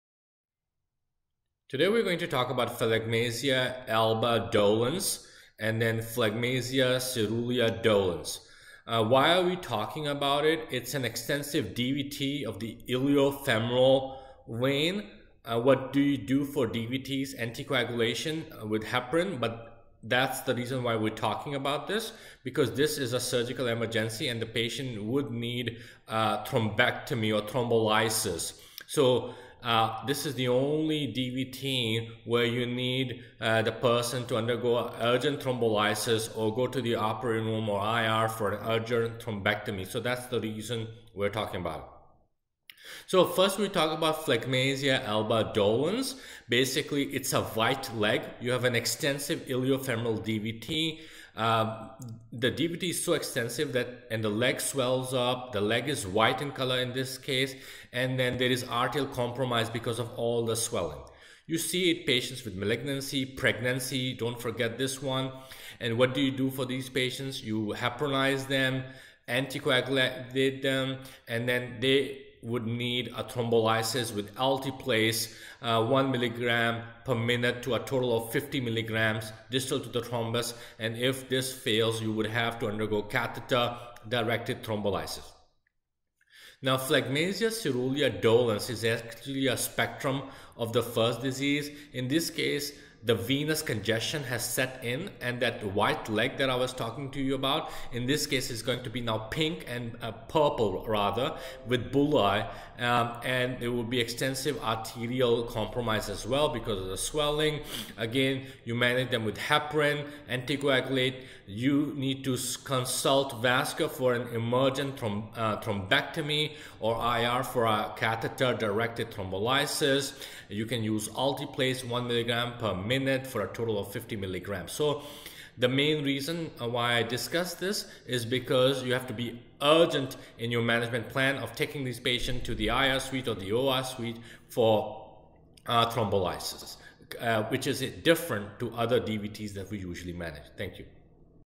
Phlegmasia Alba Dolens and Phlegmasia Cerulea Dolens
Treatment of phlegmasia alba dolens (PAD) or phlegmasia cerulea dolens (PCD) should be initiated as soon as the diagnosis is suspected.

Early diagnosis and prompt treatment is crucial
for limb salvage.

There is an arterial compromise of the limb because of the extreme swelling from the DVT

There are two primary options for thrombus removal, that
is, venous thrombectomy or endovascular methods, such as
catheter-directed thrombolysis, mechanical thrombectomy,
or pharmacomechanical thrombectomy.

If symptoms progress to venous gangrene, the risk of amputation is between 20%-50% with a high rate of mortality at 20% to 40%.